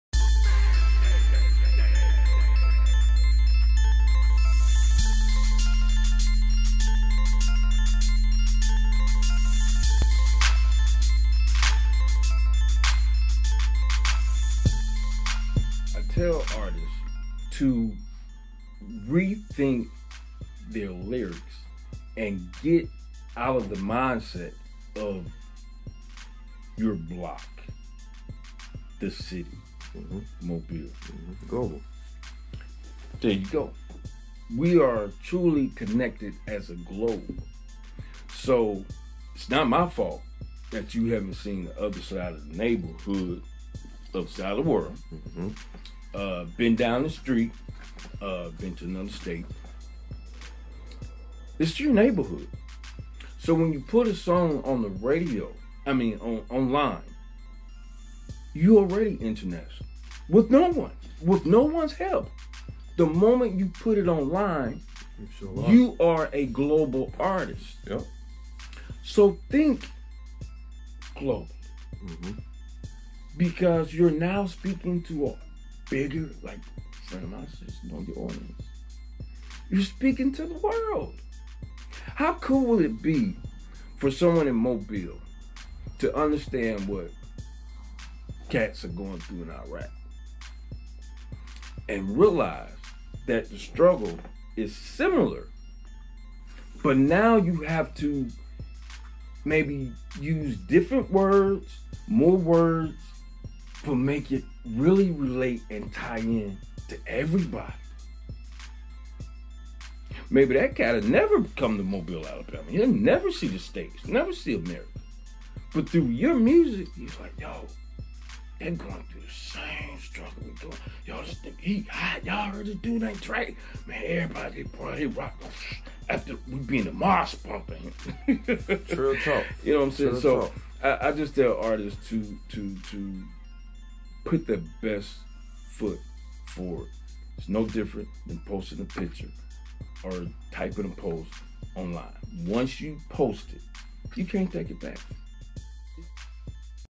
Trill Talk: Making Your Music Global With DJ Blayze
Trill Talk with your host @CeoCoo504 chopping it up with DJ Blayze. He breaks it down on why you need to make your music global.

Prints featuring Cuba, Mardi Gras and the Gulf Coast are available